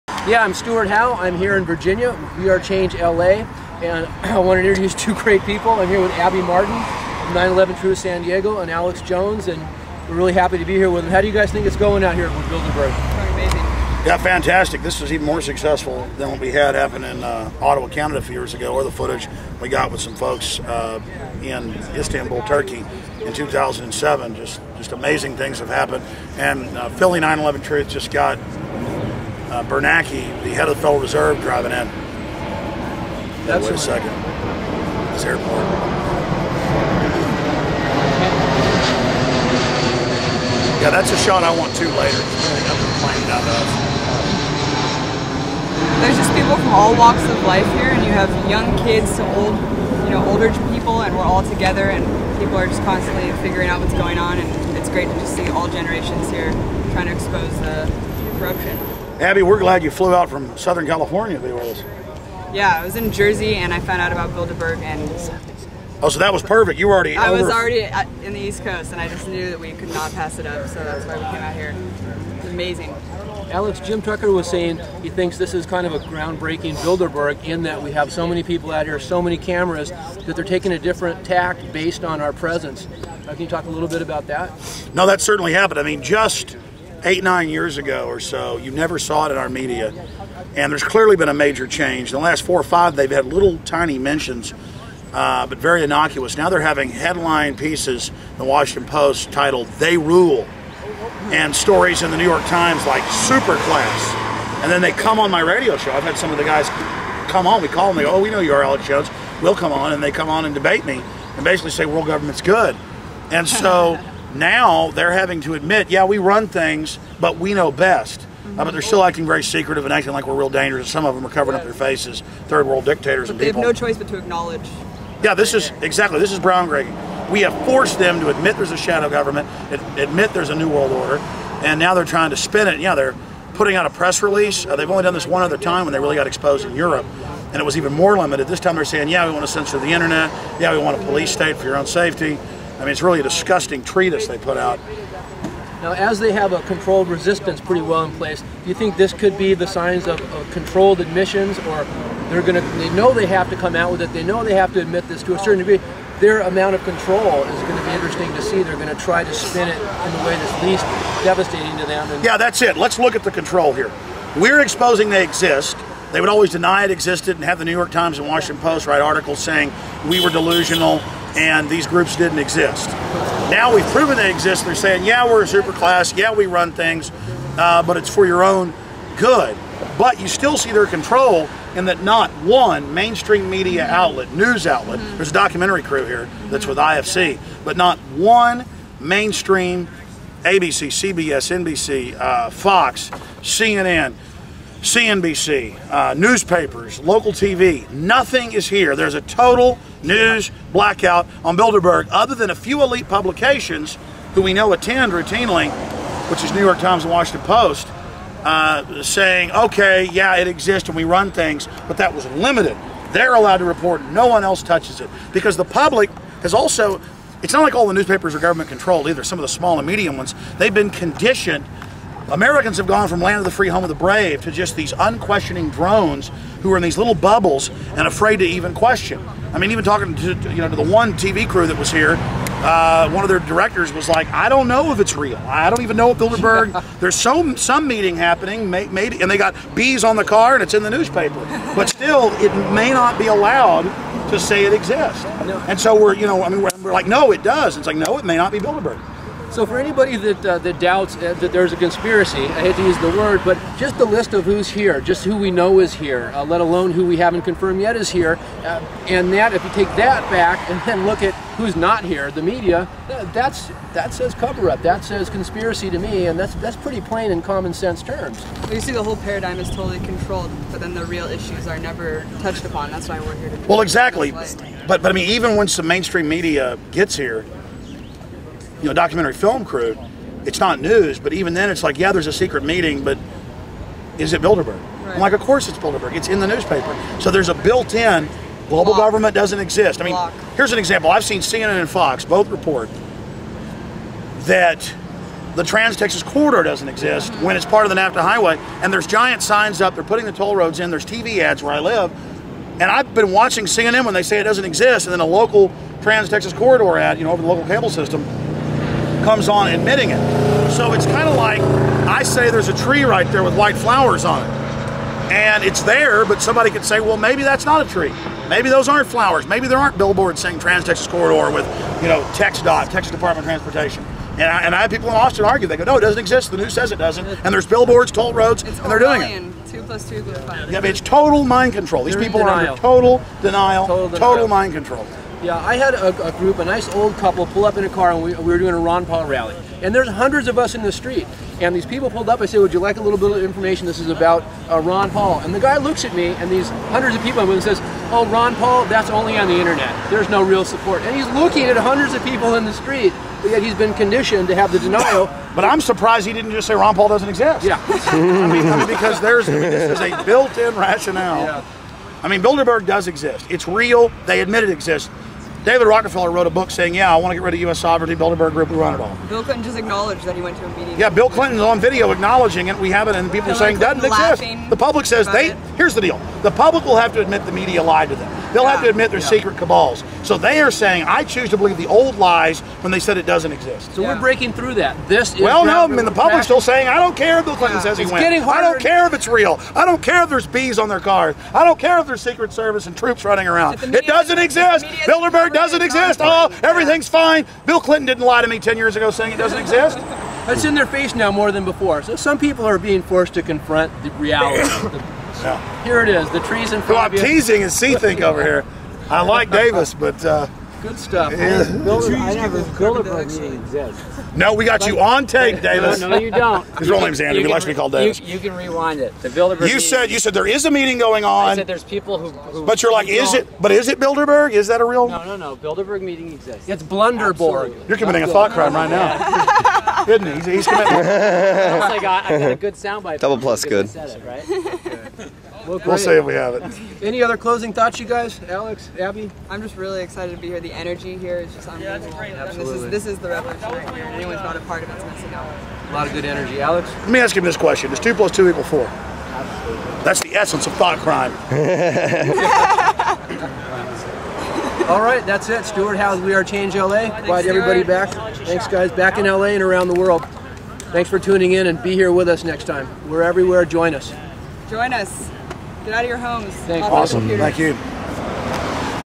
WeAreChangeLA interviews Alex Jones at Bilderberg 2008
WeAreChangeLA's Stewart Howe stands in solidarity with Abby Martin of truther.org, and Alex Jones of infowars.com at Bilderberg 2008 in Virginia outside the Westfields Marriott Hotel.